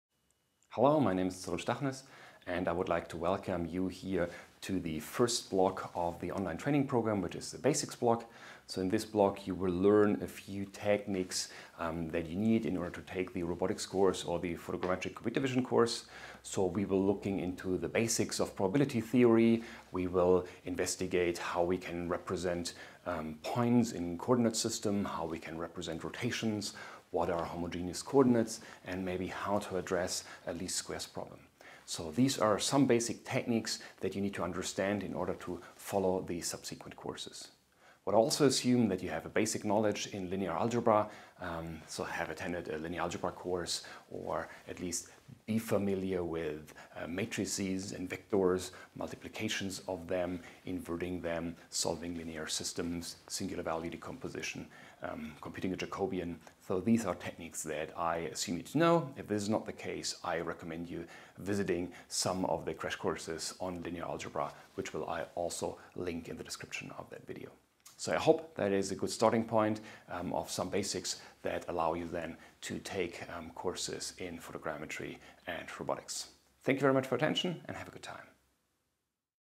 Online Training: Basics Block - Welcome (Cyrill Stachniss, 2020)
A short welcome to the Basics Block of the Online Training
Cyrill Stachniss, Fall 2020